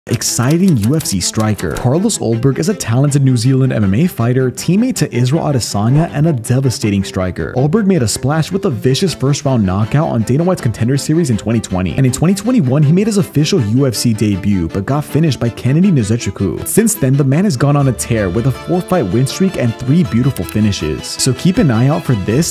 Will Carlos Ulberg Become UFC Champion | Carlos' Ulberg Flashy UFC Carer So Far
"Exciting UFC striker

Carlos Ulberg is a talented New Zealand MMA fighter, teammate to Israel Adesanya, and a devastating Striker.

Ulberg made a splash with a vicious first round knockout on Dana White’s Contender Series in 2020.

And in 2021 he made his official UFC debut but got finished by Kennedy Nzechukwu.

But since then, the man has gone on a tear, with a 4-fight win streak and 3 beautiful finishes,

So keep an eye out for this